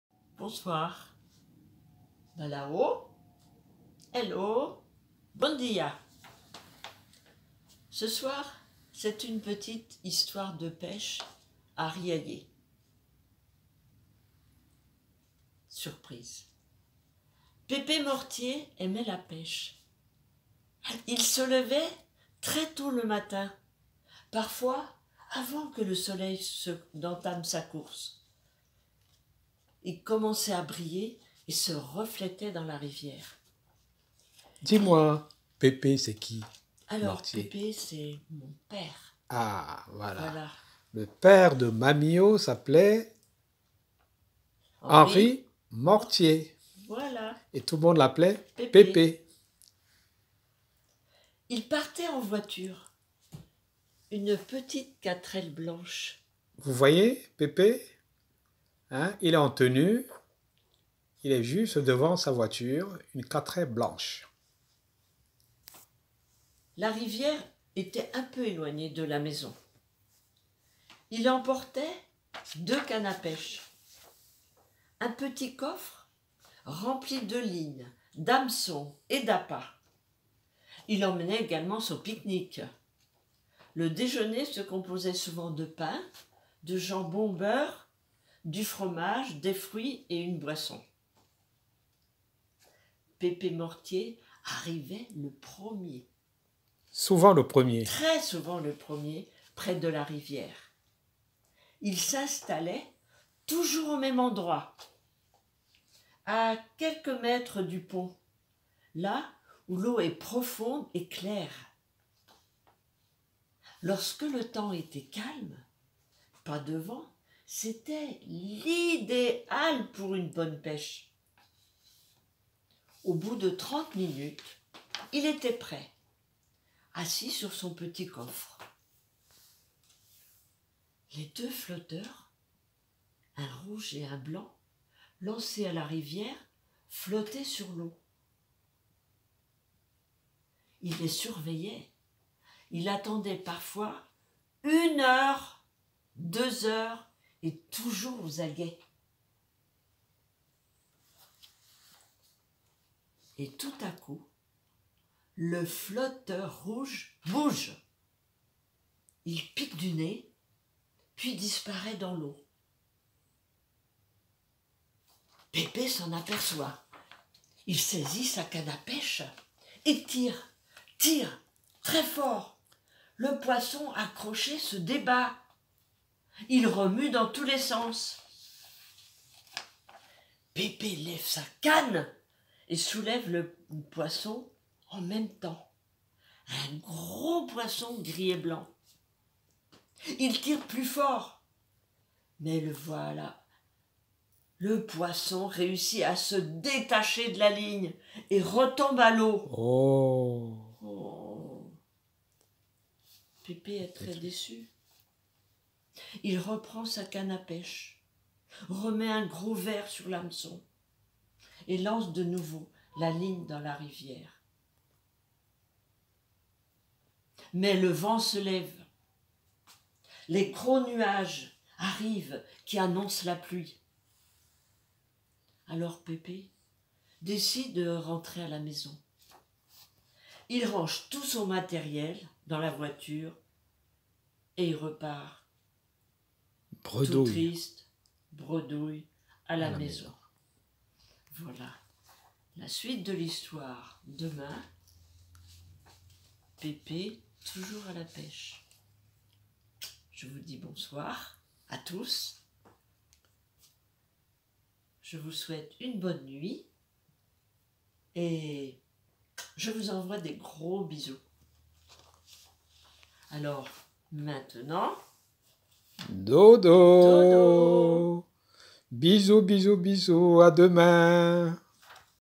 Babou raconte "Une petite histoire de pêche à Riaillé" partie 1/2
Papi Mortier se prépare pour son activité préférée... la pêche! Nous allons suivre ses pas! :)